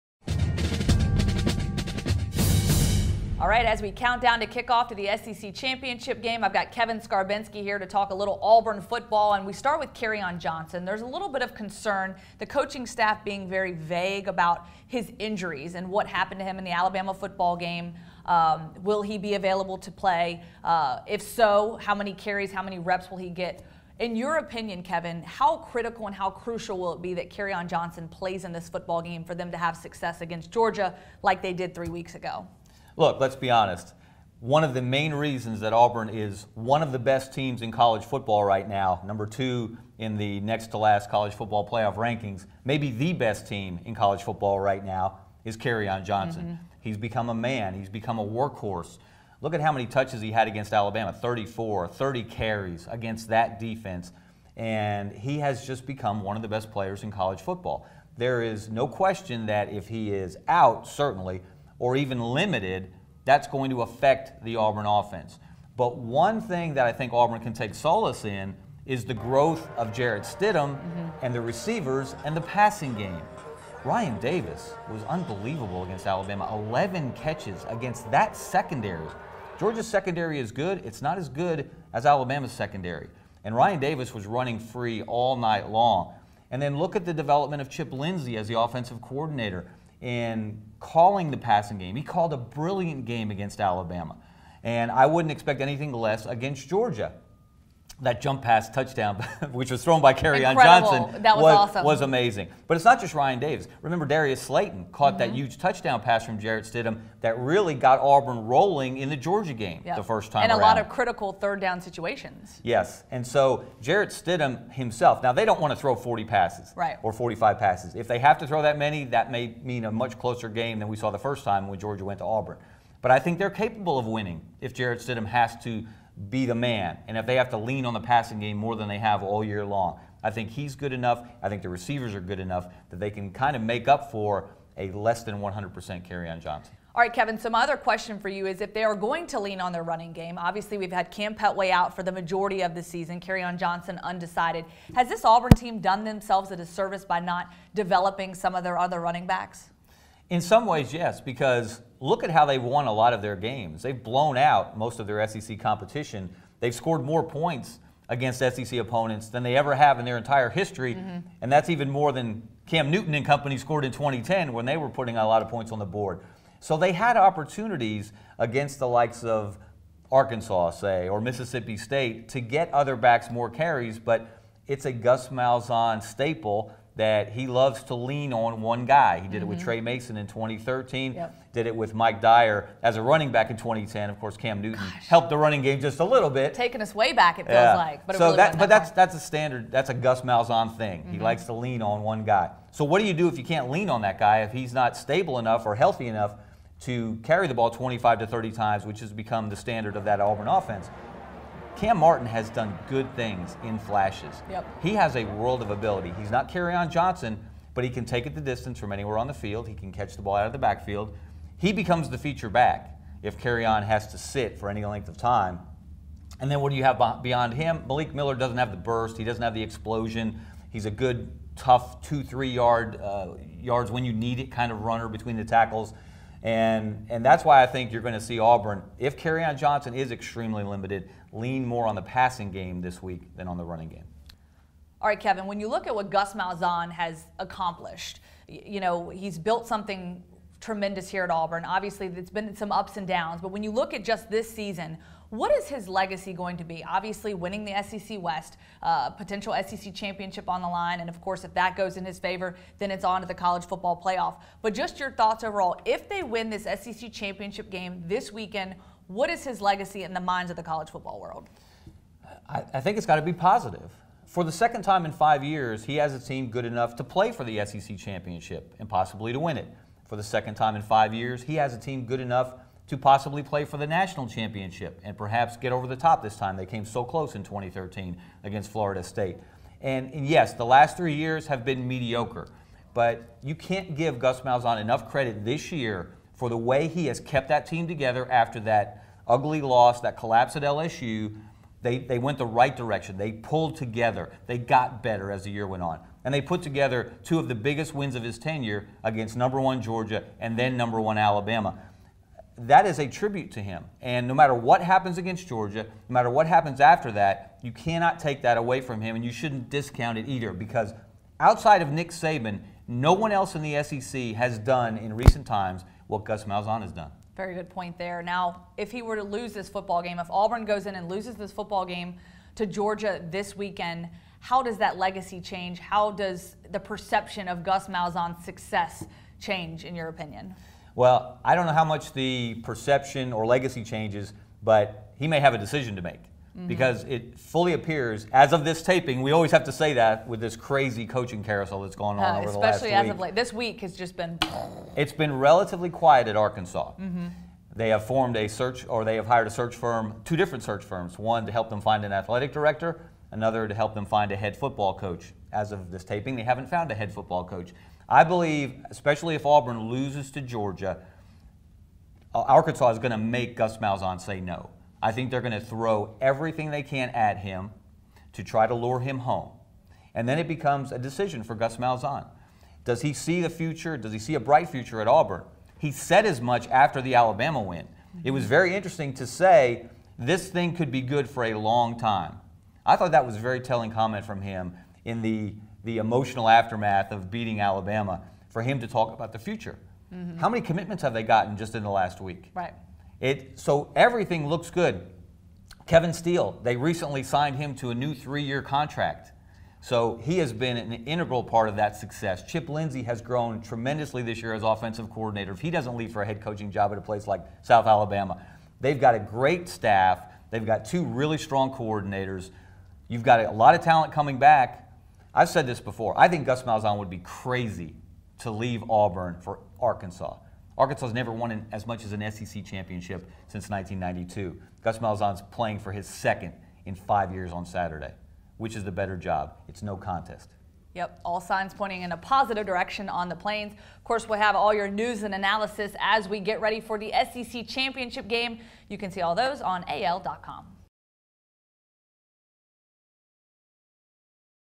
Can Auburn beat Georgia without a healthy Kerryon Johnson?
Auburn faces Georgia in the SEC Championship game on Saturday and will potentially be without it’s top running back Kerryon Johnson. AL.com’s Kevin Scarbinsky and Lauren Sisler discuss the impact this could have on the outcome of the game and the adjustments the Tigers will need to make. They also talk about Gus Malzahn’s track record at Auburn and what a win or a loss this weekend can mean for his future.